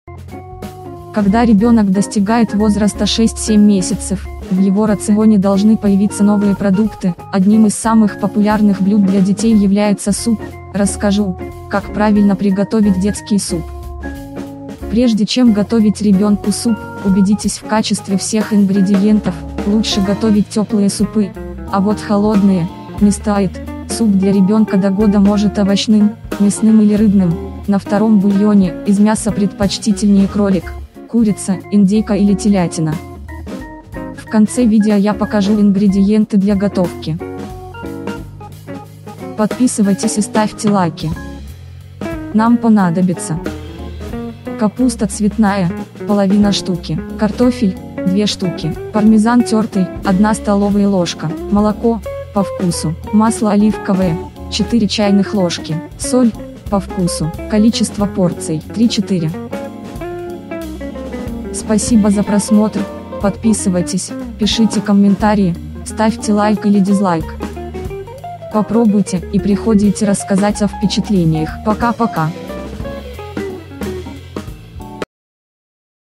★UNREAL!★ ДЕТСКИЙ СУП ДО ГОДА ★
★Нельзя не приготовить● Когда ребенок достигает возраста 6-7 месяцев, в его рационе должны появиться новые продукты● Одним из самых популярных блюд для детей является суп● Расскажу, как правильно приготовить детский суп●